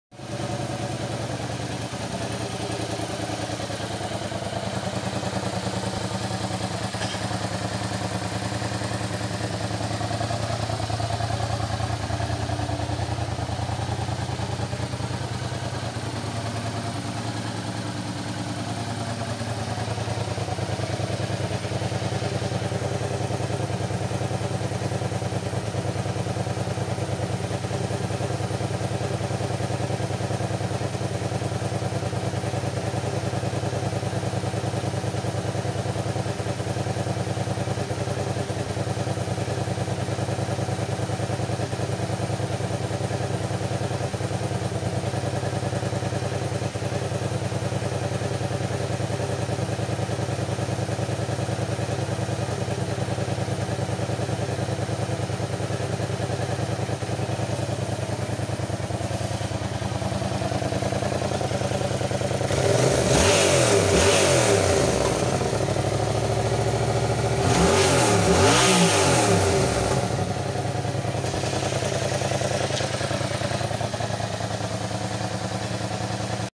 mt07 ho ( high output) Yamaha motter bike
my mt07 engine running exhaust noise high out put engine.